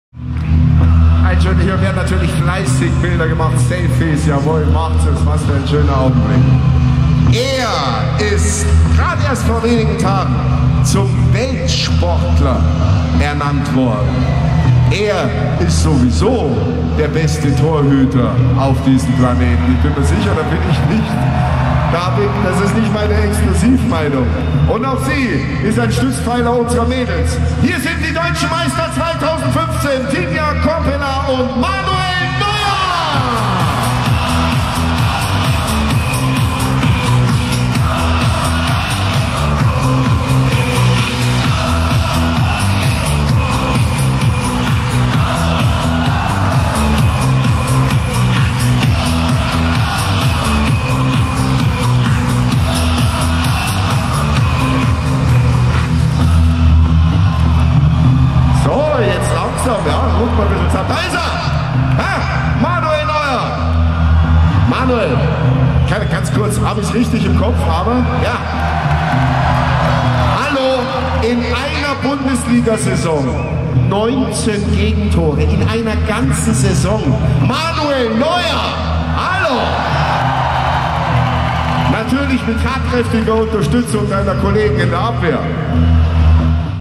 Manuel Neuer lässt es krachen - Meisterfeier FC Bayern 2015
FC Bayern-Torwart Manuel Neuer hat bei der Meisterfeier auf dem Balkon des Rathauses in München den Emotionen freien Lauf gelassen.